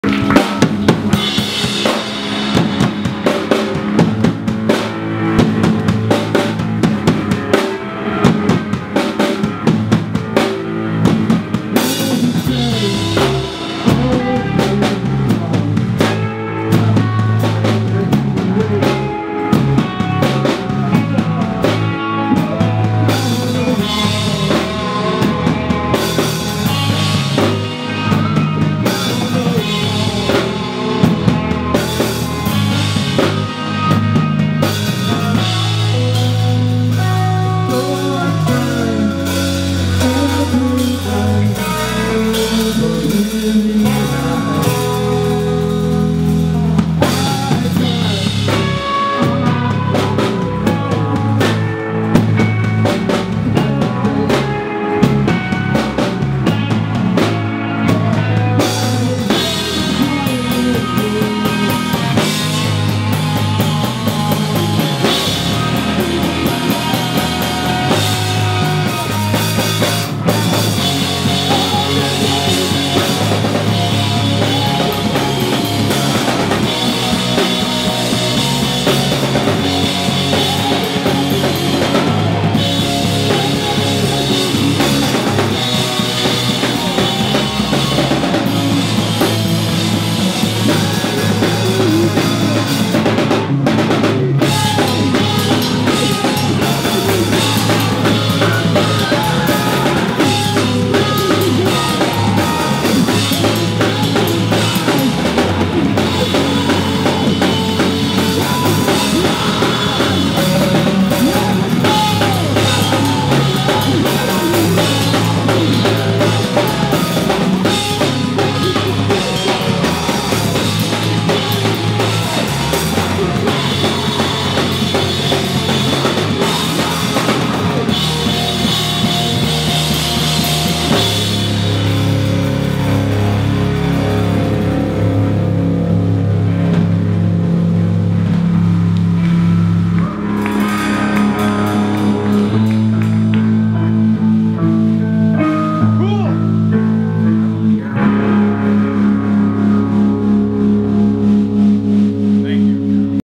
Raw Blow - song 4
Raw Blow performing during night 1 of Kat Kat Records Phest Two on December 28th, at The Golden Tea House in Philadelphia, Pa.